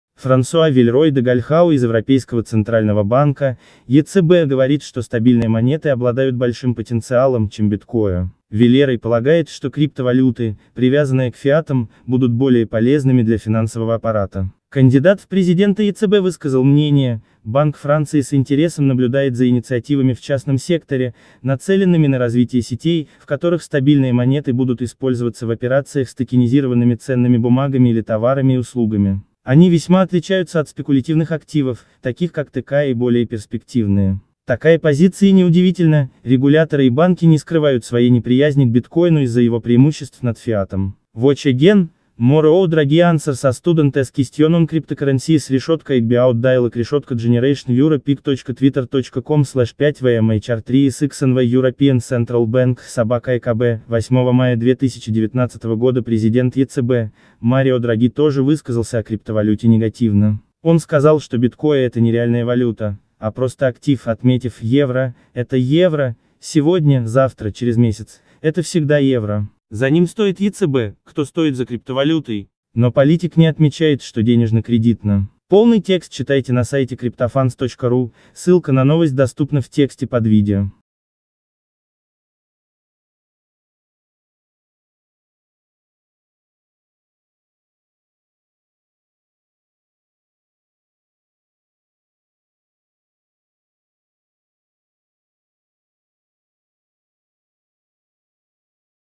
Европейский центральный банк: стейблкоины лучше, чем биткоин.
Франсуа Вильрой де Гальхау из Европейского центрального банка (ЕЦБ) говорит, что стабильные монеты обладают большим потенциалом, ч